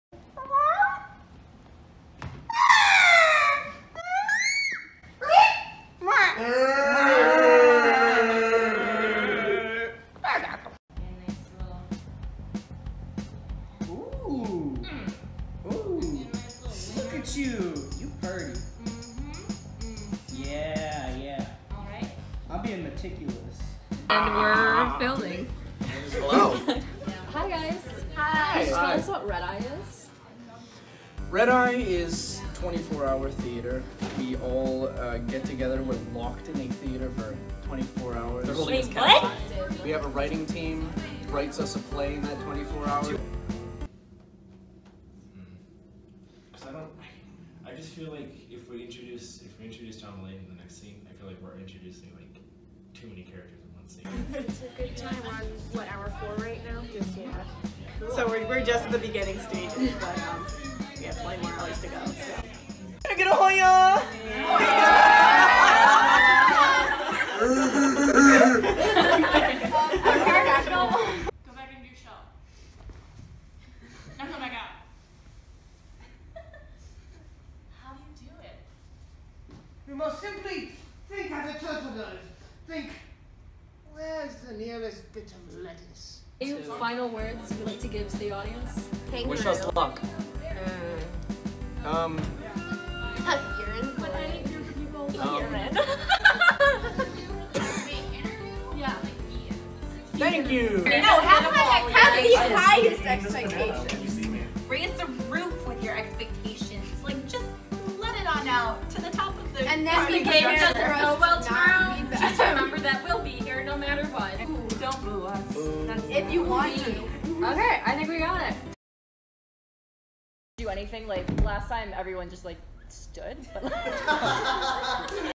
Red Eye Fall 2018 Producer Sketch
Our talented writers, actors, and directors have stayed up all night creating more ridiculous theater for your entertainment. Enjoy this behind-the-scenes look at the making of the mayhem.